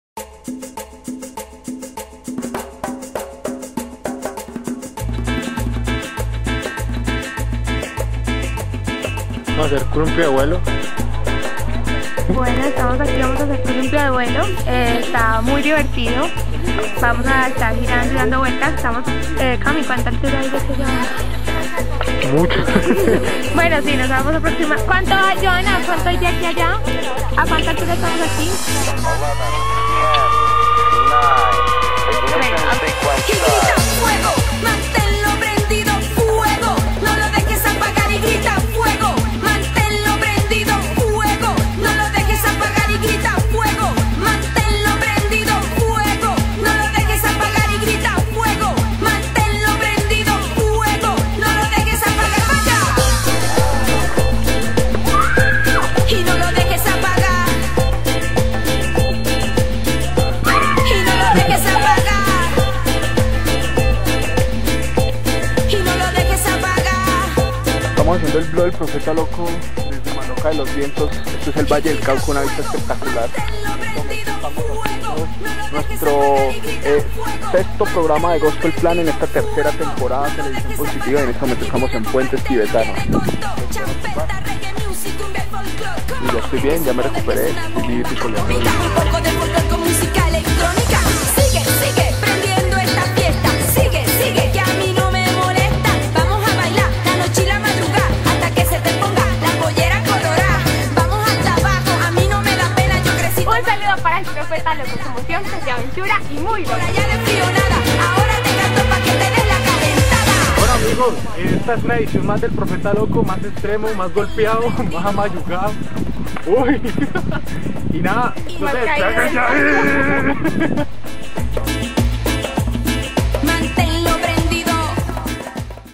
6 de Marzo 2011
No son Deportes Extremos . Son deportes de AVENTURA .. EStuvimos en MALOCA de los vientos ¡¡¡¡¡ Mira como la pasamos